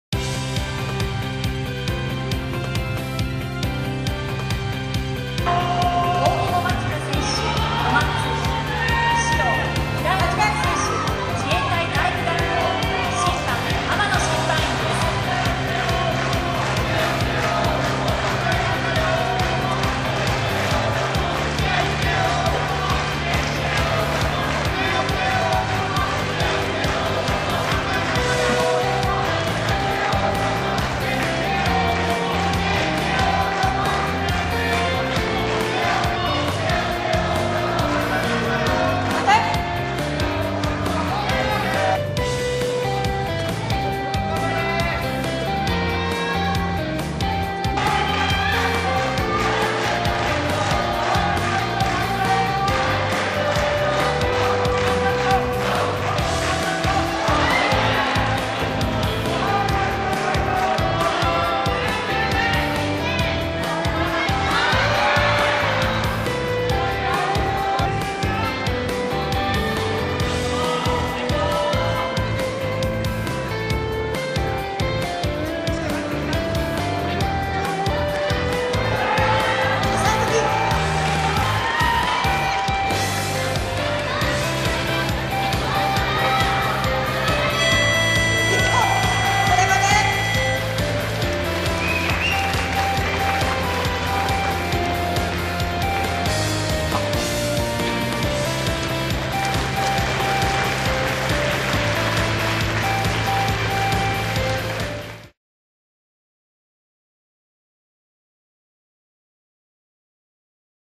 【57kg級決勝 大友vs平井】平成25年全日本選抜柔道体重別選手権大会｜柔道チャンネル
平成25年全日本選抜柔道体重別選手権大会57kg級決勝の大友vs平井をご覧頂けます。

■平成25年全日本選抜柔道体重別選手権大会